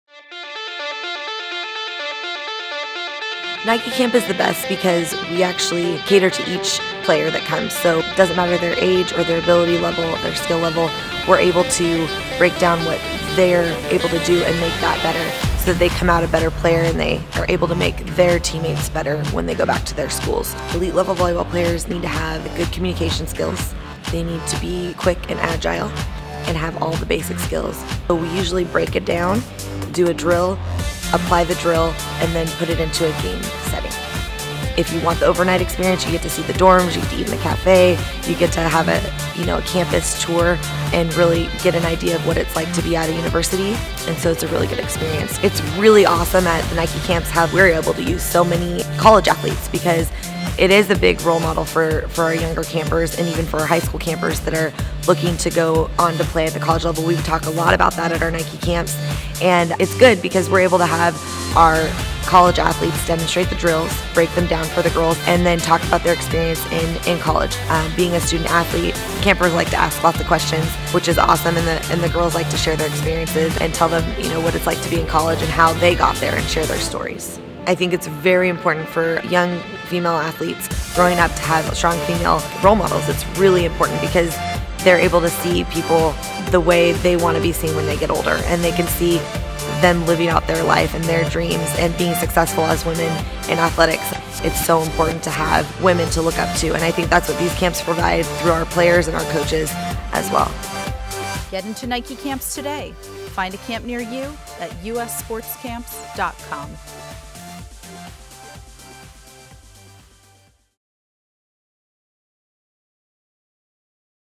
Nike Volleyball Camps Info Video 2016
Nike Volleyball Camp Director and William Jessup Head Volleyball Coach, Shasta Millhollin, explains what it means to experience a Nike Volleyball Camp. A complete schedule of our dates and locations can be found